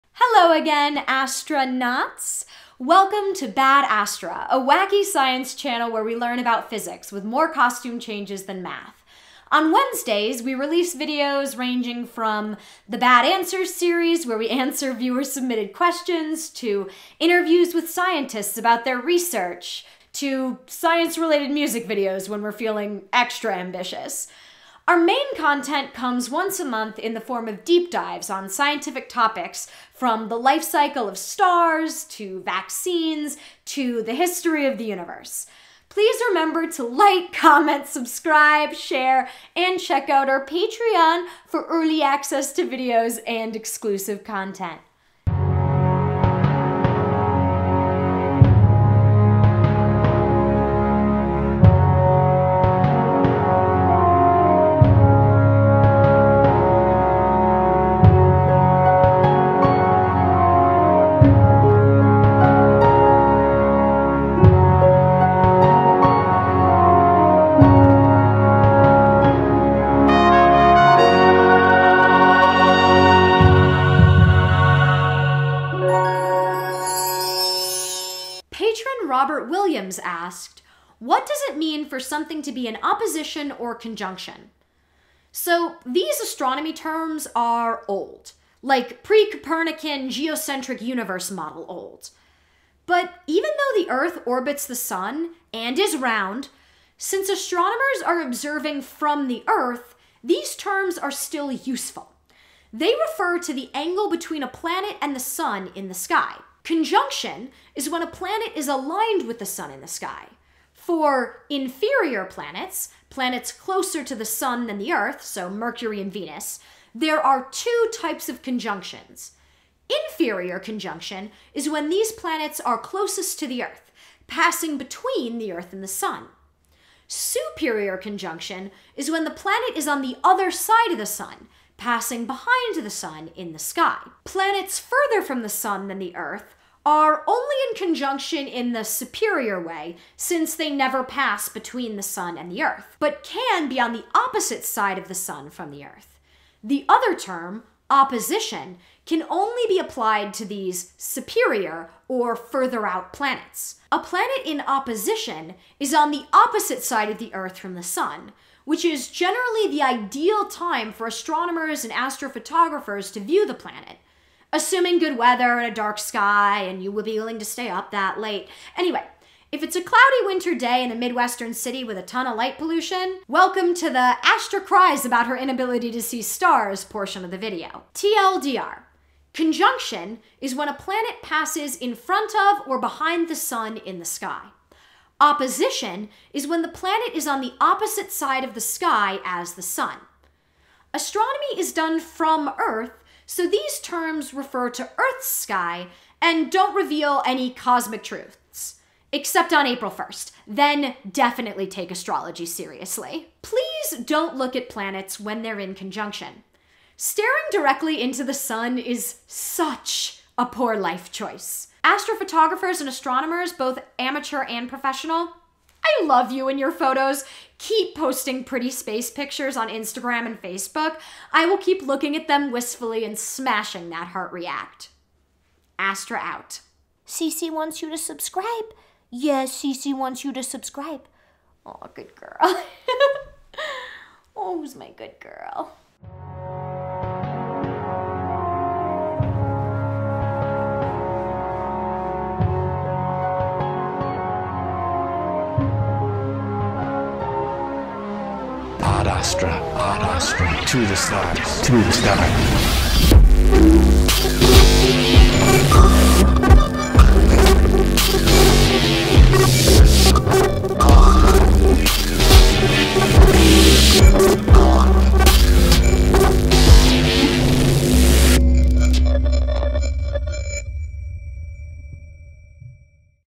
Opposition vs. Conjunction
What does it mean for something to be in opposition or conjunction? So these astronomy terms are old. Like, pre-Coperican, geocentric universe model old.
TL;DR: ""Conjunction"" is when a planet passes in front of or behind the Sun in the sky, and ""Opposition"" is when the planet is on the opposite side of the sky as the sun. Astronomy is done from Earth, so these terms refer to the Earth's sky, and don't reveal any cosmic truths, except on April 1st.